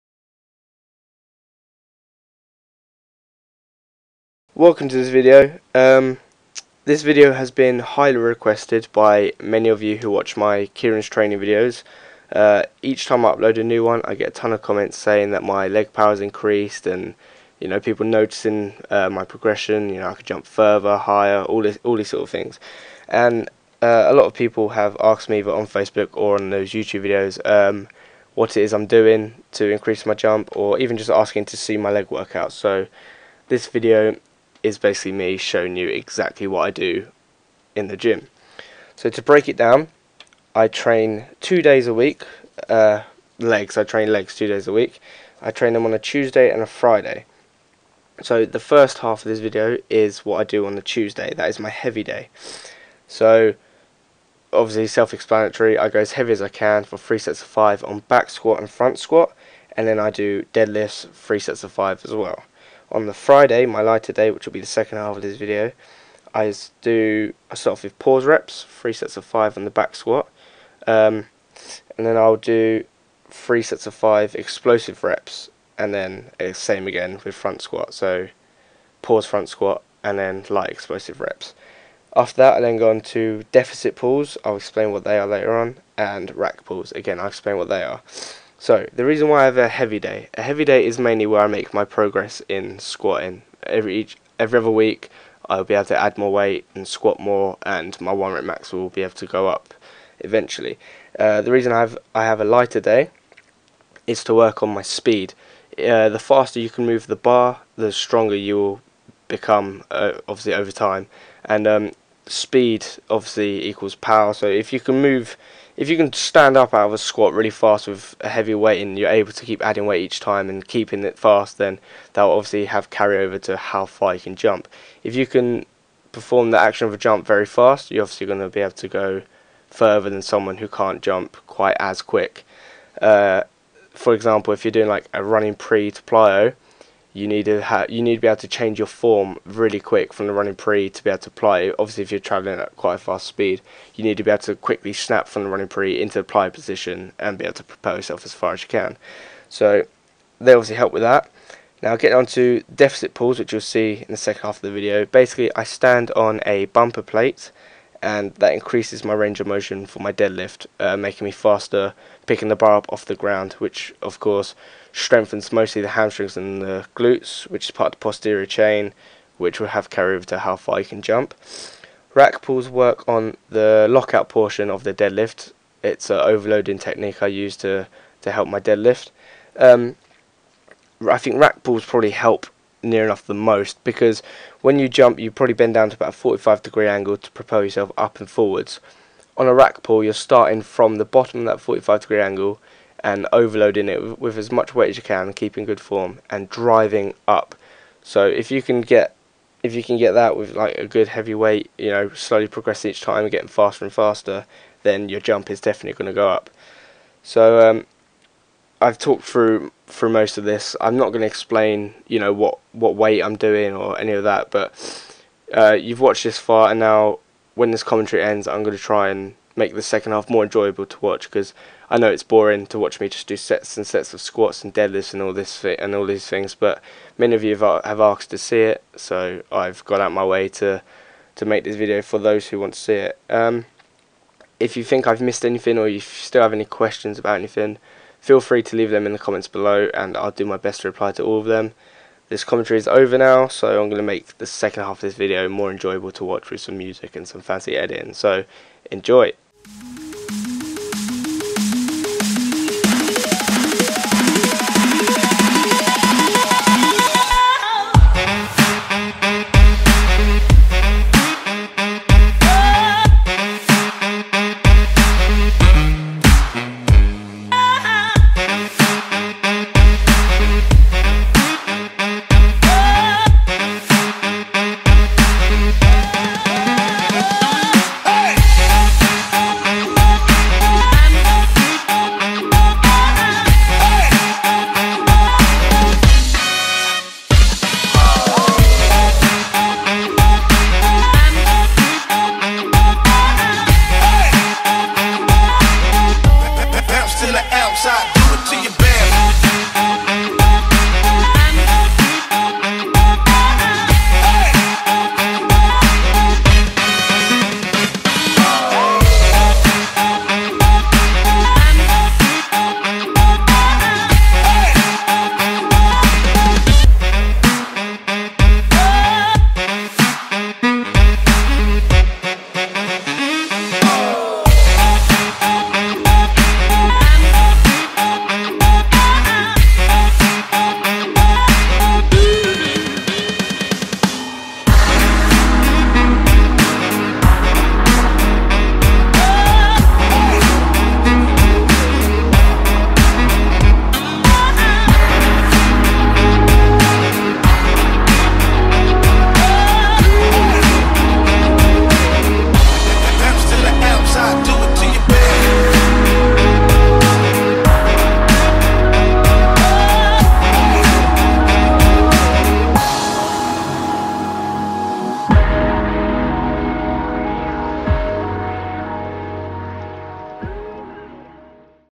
'How I Increase My Jump' Episode 301
highly requested video! im sorry i mumble lots and i find it hard to explain what i mean by using just my voices (i can describe better using actions and stuff). i really hope this helps any of you who are thinking about strength training to improve your jump or overall wanna better the condition of your legs! feel free to leave any questions below! happy to help!